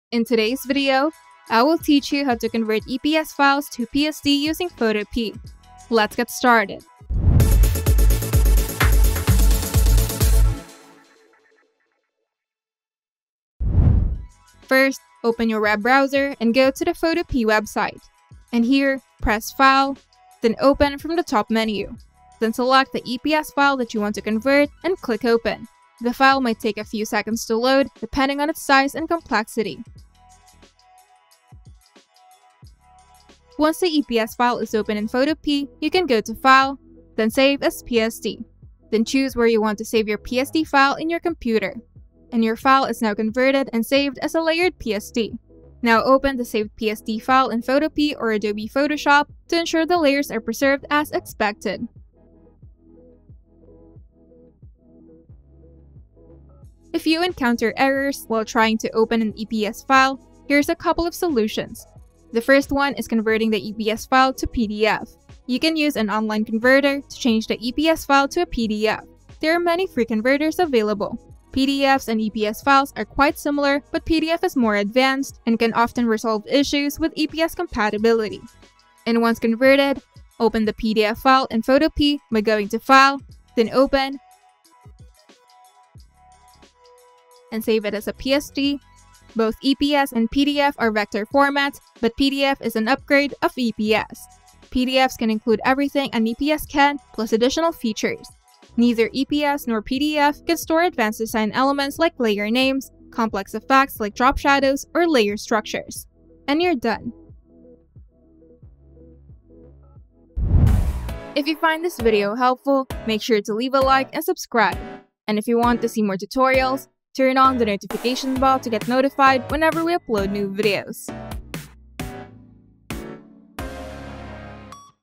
How to Convert EPS Files to PSD Using Photopea (Adobe Illustrator Vector File to Photoshop File)
Need to convert EPS files to PSD without Adobe Illustrator? Learn how to use Photopea to open, edit, and save EPS files as PSD for seamless Photoshop compatibility.